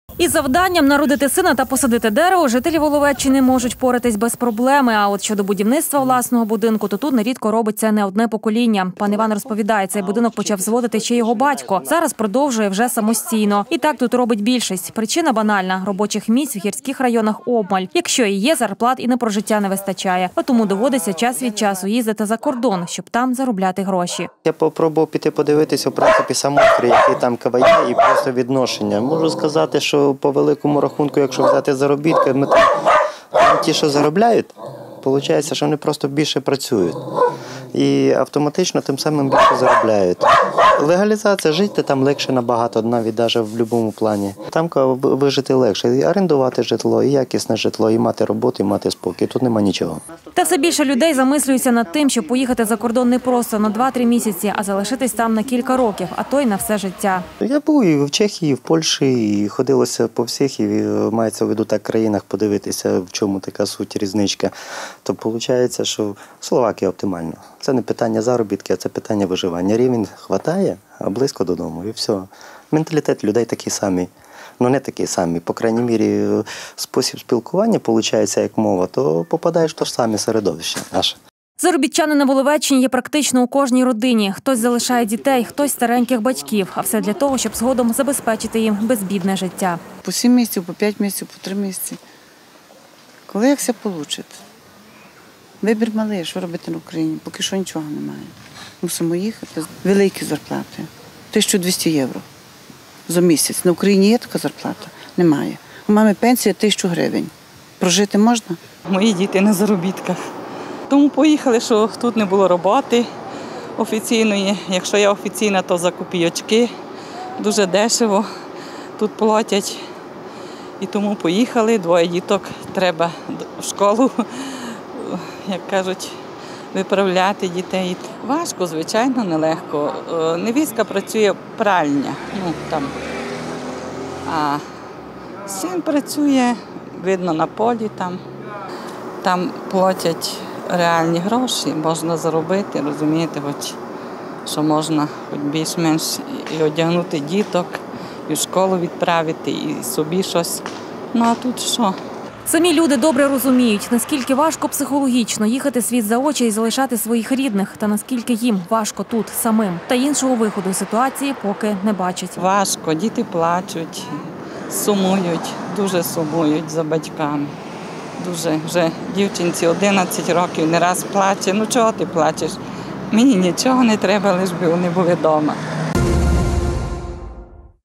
За кордон – на заробітки
За кордон на заробітки їдуть більше половини жителі Закарпаття. До прикладу, у Воловці місцеві кажуть, в кожній родині тут є заробітчани, бо інакше тут не вижити. Робочих місць обмаль, а якщо і є, то зарплати – копійчані. Куди переважно їздять закарпатці, де шукають кращої долі – дивіться далі.